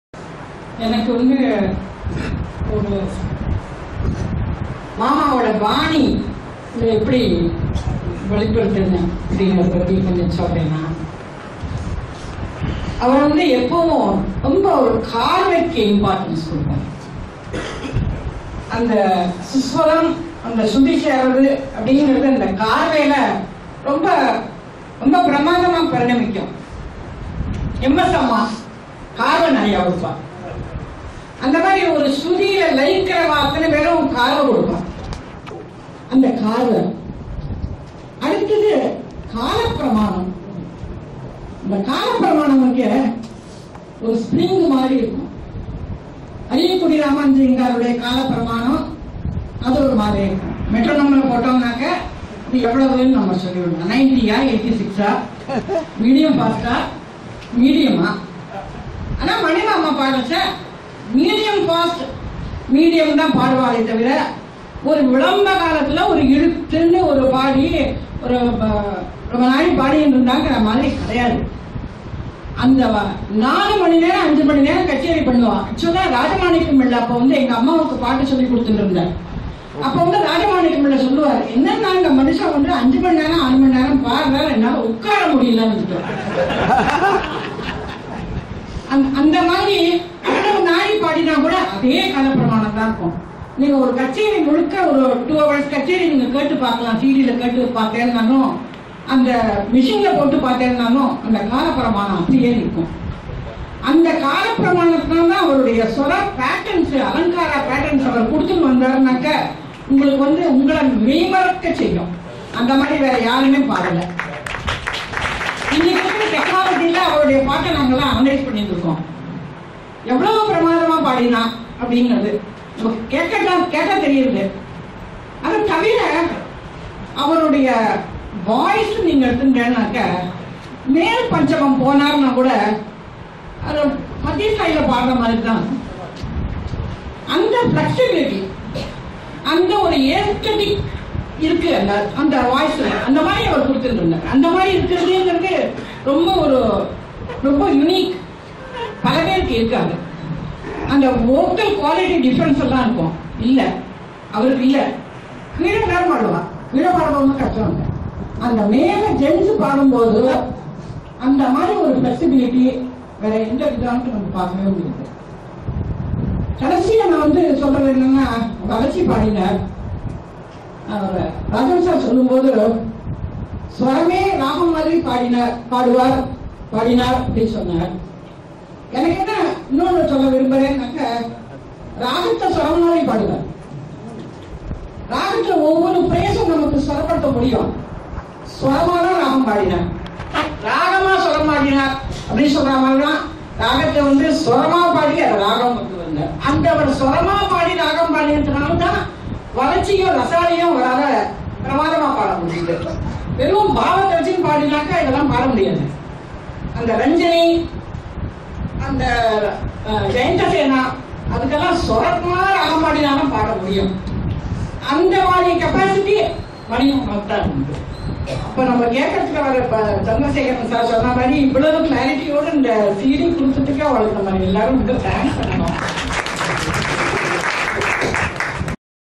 Dr.S.A.K. Durga on Mani Iyer
As a Tribute to Dr. S.A.K. Durga, the acclaimed Musicologist and a disciple of Madurai Mani Iyer who passed away recently, here is an excerpt of her talking about Mani Iyer. It was captured during the Madurai Mani Iyer Centenary Celebrations in 2012.